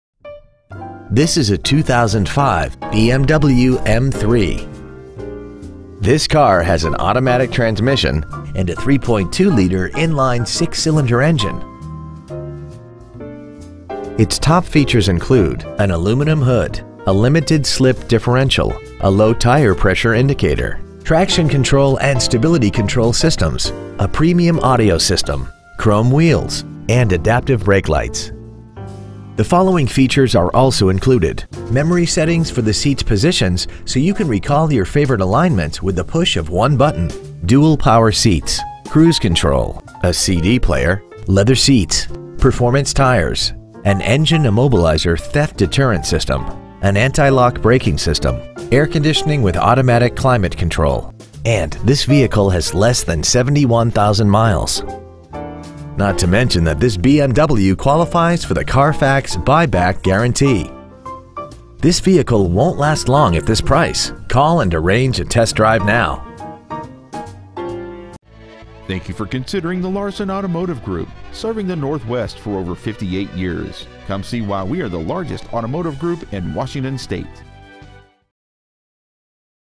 2005 BMW M3 Puyallup WA
Year : 2005
 Make : BMW
 Model : M3
 Engine : 3.2L I6
 Trans . : Automatic
 Exterior : Silver
 Miles : 69,139
 Interior : Black
 Stock : 31614A

 2005 BMW M3 Puyallup WA